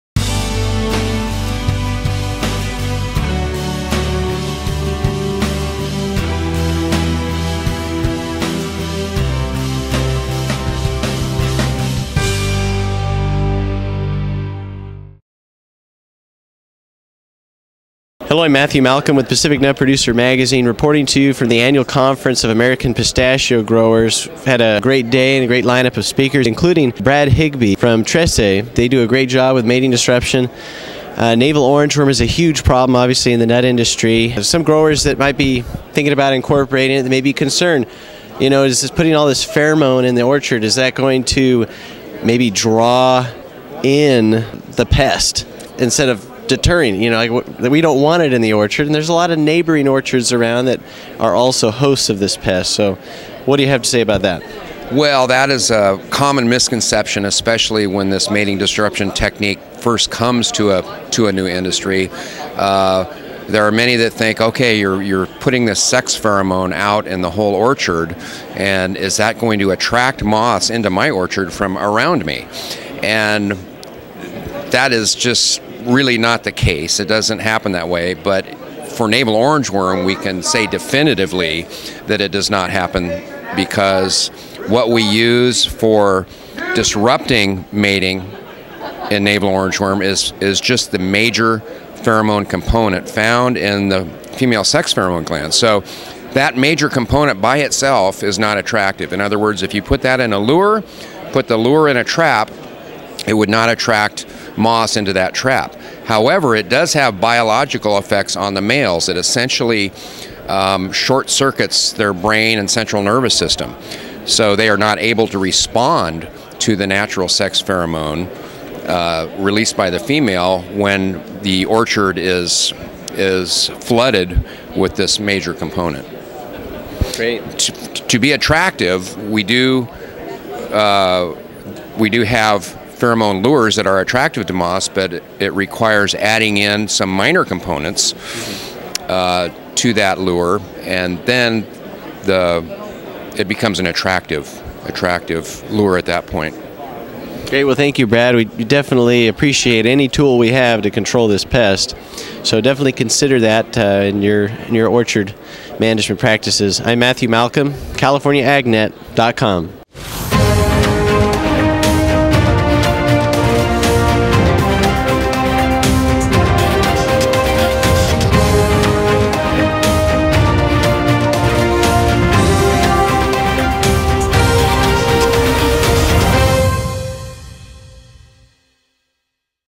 Concern for Navel Orangeworm Mating Disruption
Navel Orangeworm has become a serious pest in California for many agricultural commodities, especially for tree nuts.  Mating disruption technology has proven to be a critical tool in managing this pest; however some growers may have concerns with utilizing this technology on their farms.  Check out this brief interview with Brad Higbee, now Field Research & Development Manager for Trece, Inc. as he works with growers to overcome concerns and clear up any misconceptions regarding this novel technology in the nut industry.  Read more about it in Pacific Nut Producer Magazine.  Don’t currently receive the magazine?